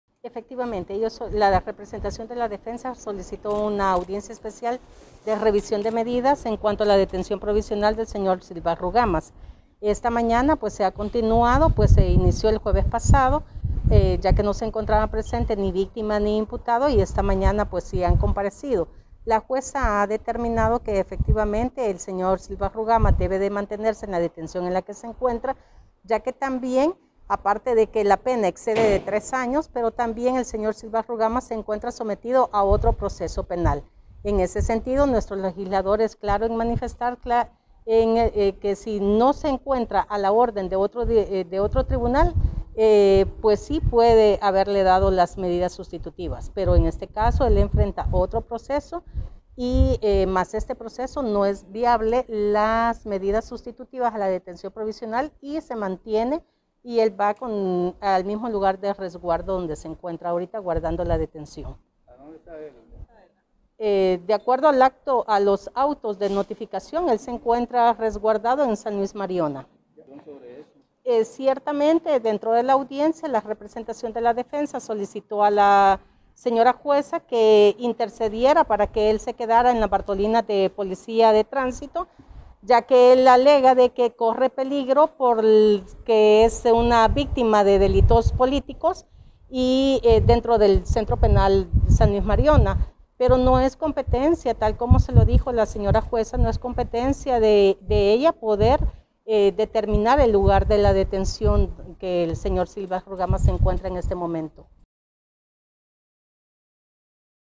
Youtuber Roberto Silva seguirá detenido en Mariona
Silva pidió que lo trasladaran de "Mariona" pero la jueza le dijo que no porque no era su jurisdicción

¡Visítenos!